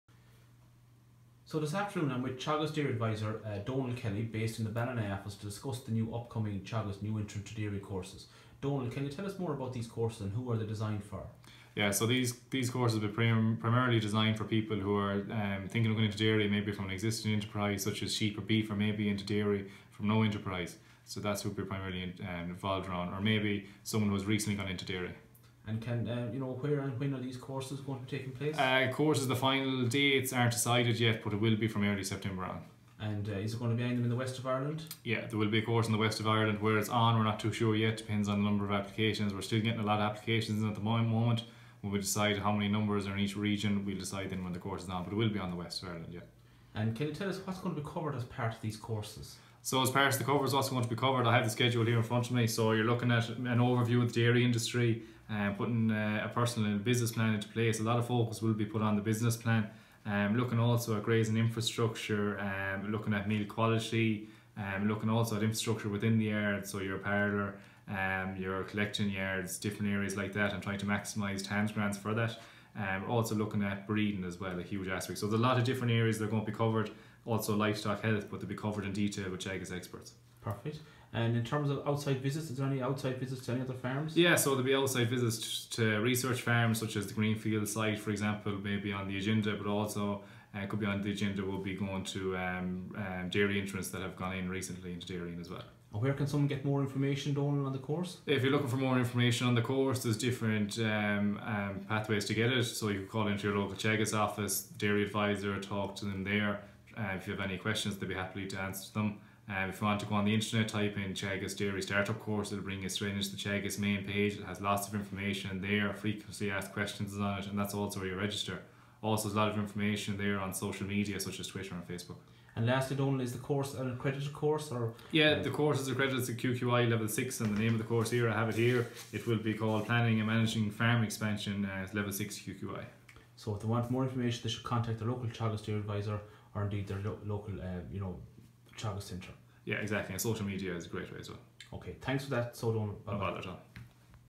New Entrant to Dairy course video 2018
Short video produced featuring Teagasc Dairy Advisor Donal Kelly. Produced by Brendan Garry